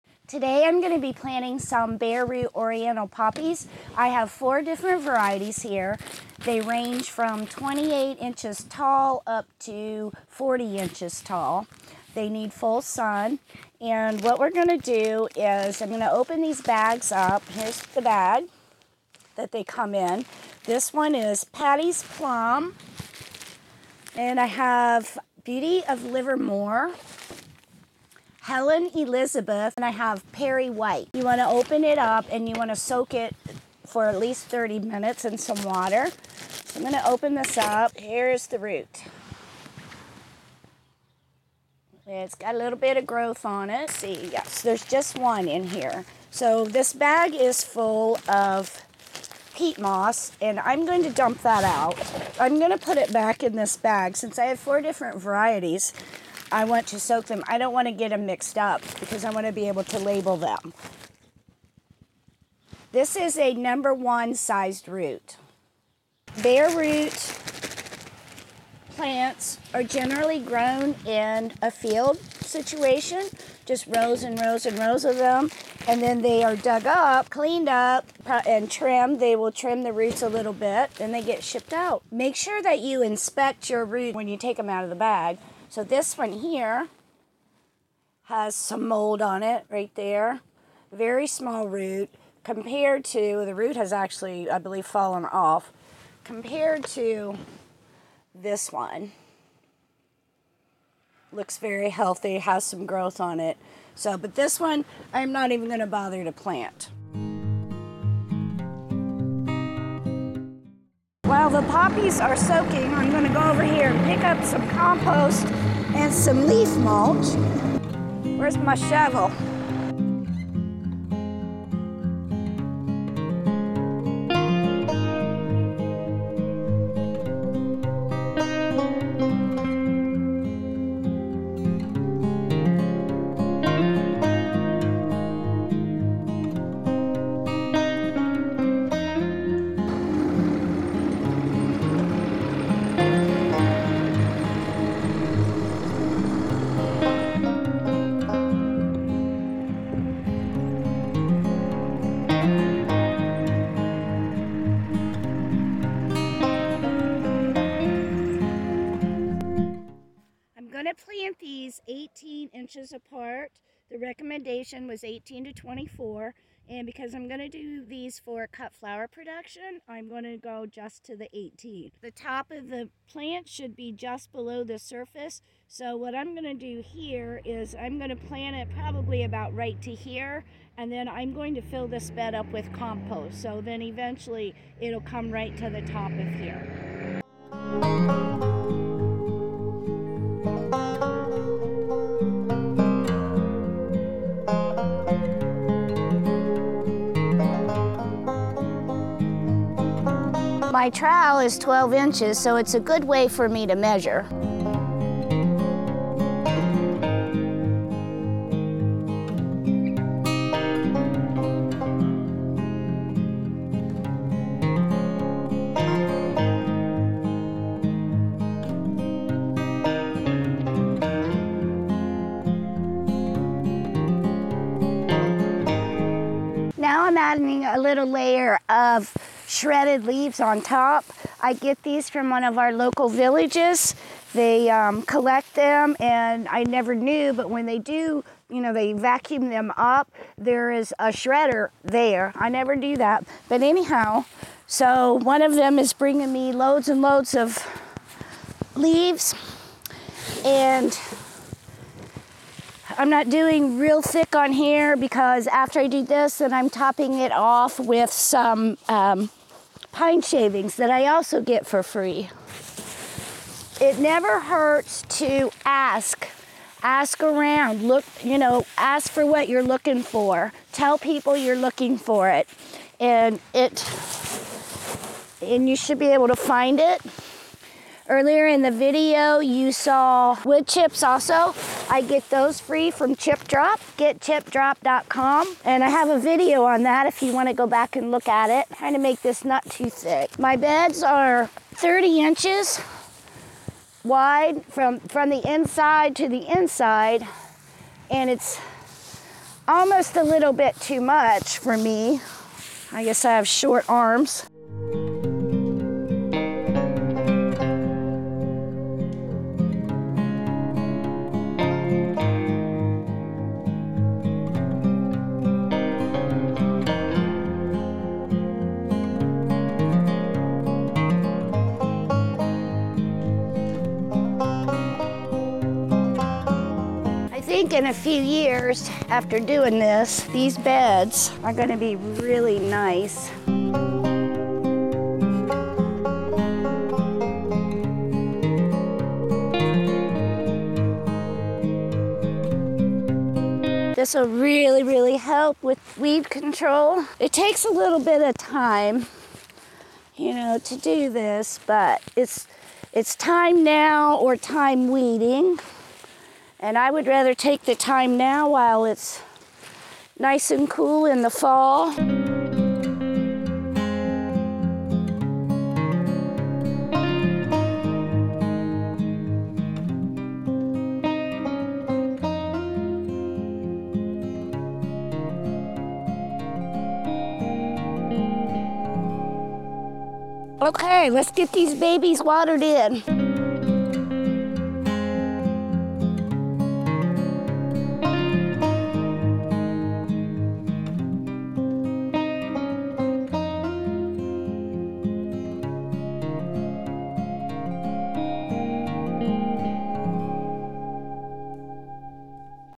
Planting Bare Root Oriental Poppies in The Fall
This video is showing how I plant Oriental poppies in late October.